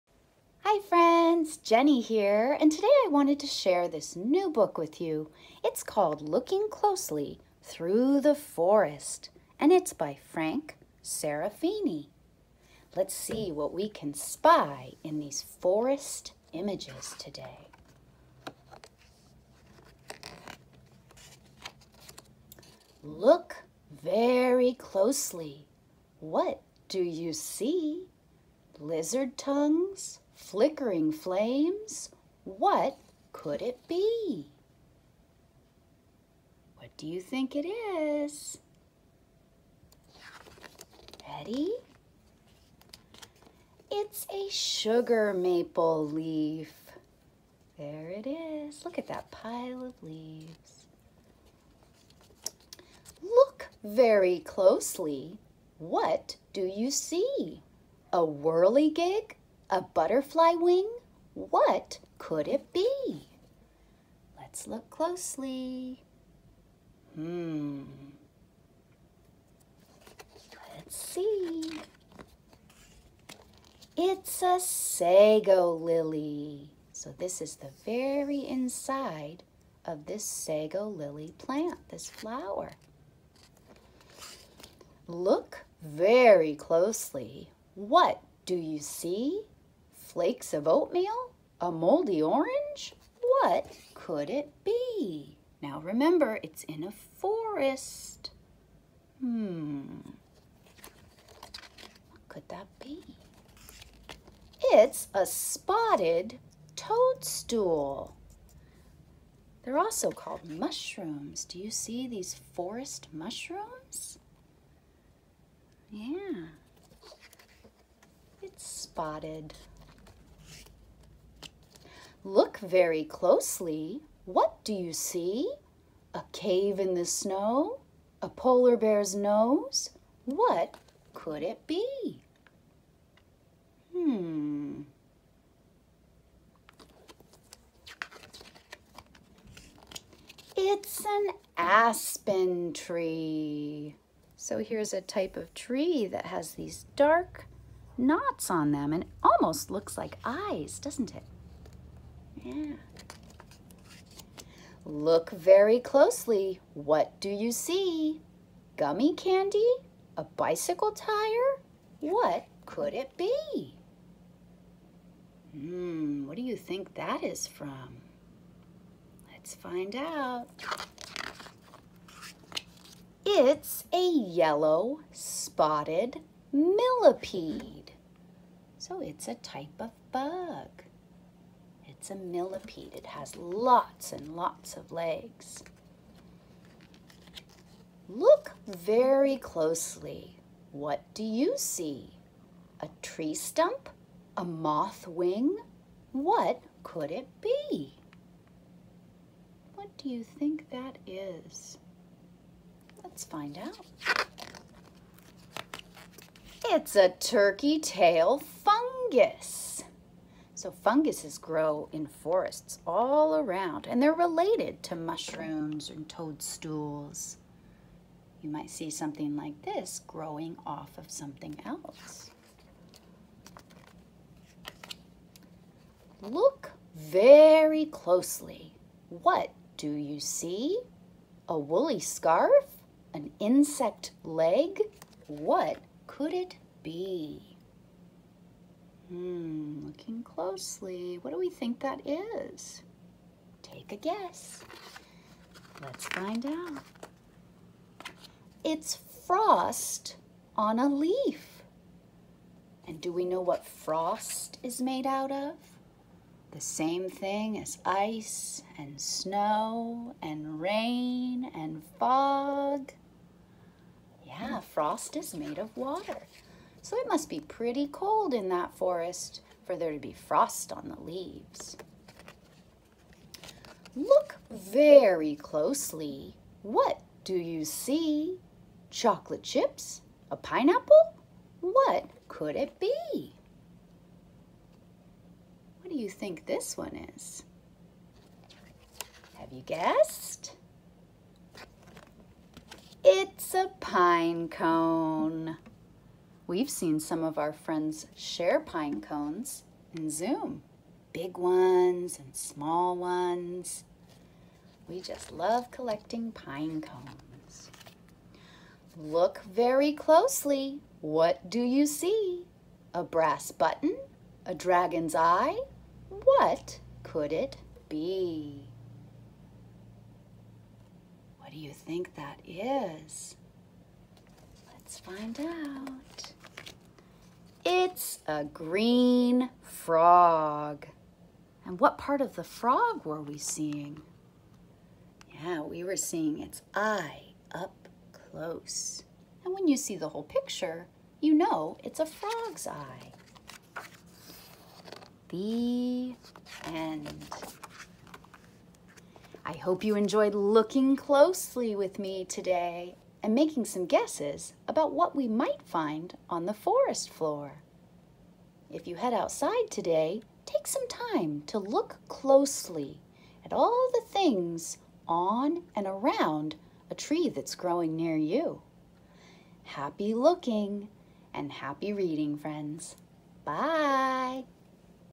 “Looking Closely Through the Forest” by Frank Serafini
As looking and guessing books have engaged this group of young children, here’s another invitation to look closely and perhaps take a guess at what it might be! This is a fun one to watch as a family as children love to see their parents stumped along with them! Frank Serafini’s “Looking Closely Through the Forest” focuses in on one habitat and shows a few familiar fall items we‘ve seen children share recently, as well as other surprising or new images to add to previous knowledge. Children may request another look to review and share their new vocabulary and knowledge!